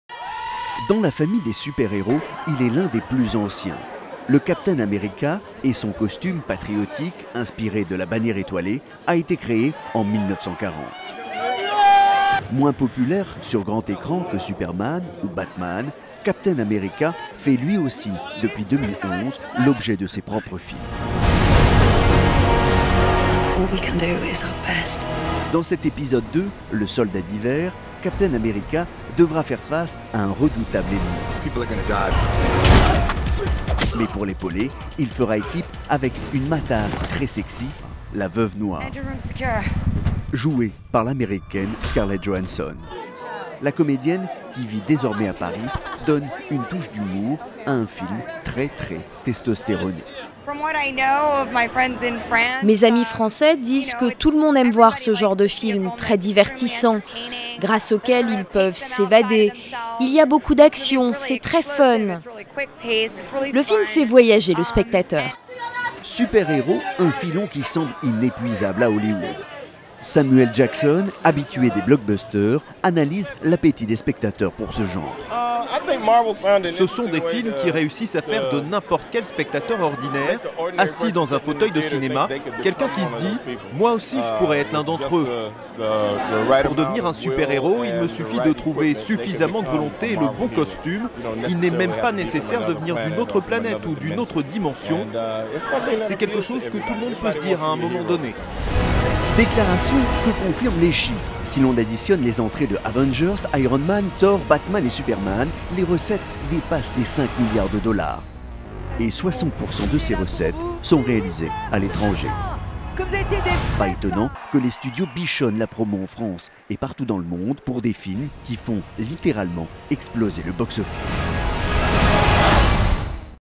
Captain America is back - Le 21/03/2014 à 22h00
Avec sa tunique bleue et son bouclier étoilé, Captain America est de retour. L'avant-première exceptionnelle de "Captain America, le soldat de l'hiver" s'est tenue lundi soir, au Grand Rex, en compagnie de toute l'équipe du film !